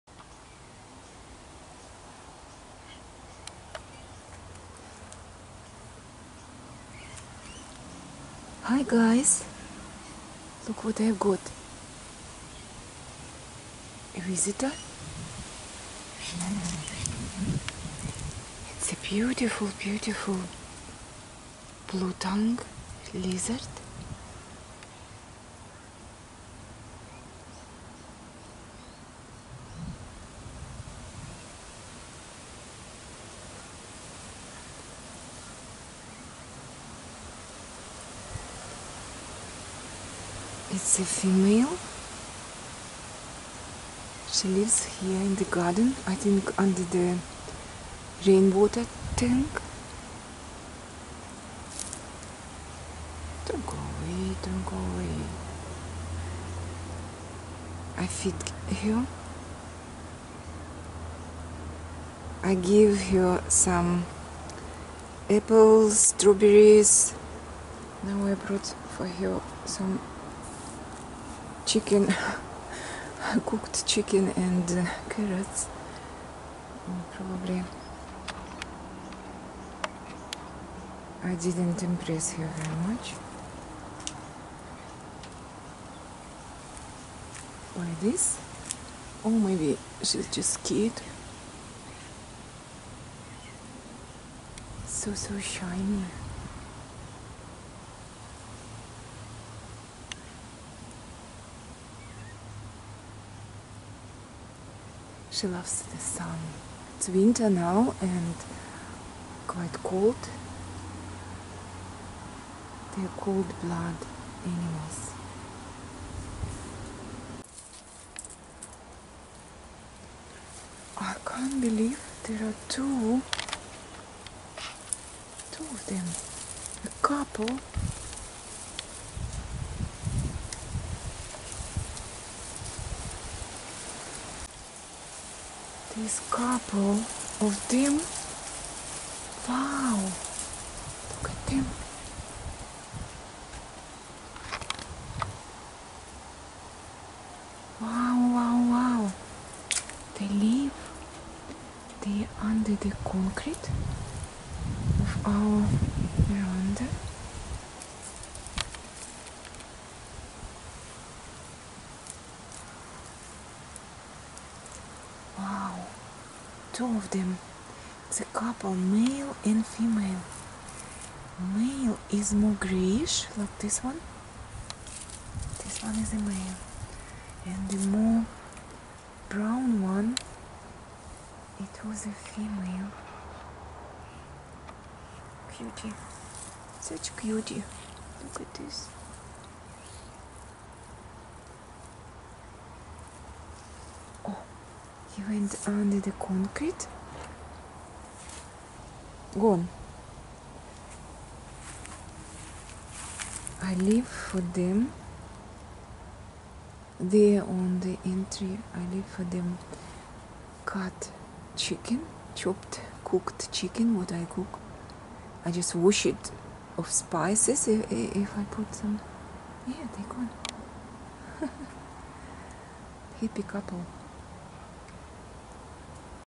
Blue Tongue In My Garden!
I haven't seen there is another one, it's a couple of dinosaurs live there ander the concrete!  I must double the portion of chicken!
Я не оджидала увидеть, что там двое ящериц живут! Они называются Голубой Язык, потому что у них действиетльно голубые языки. Эти ящерицы питаются жуками, червяками и фруктами, всем, что могут найти в саду.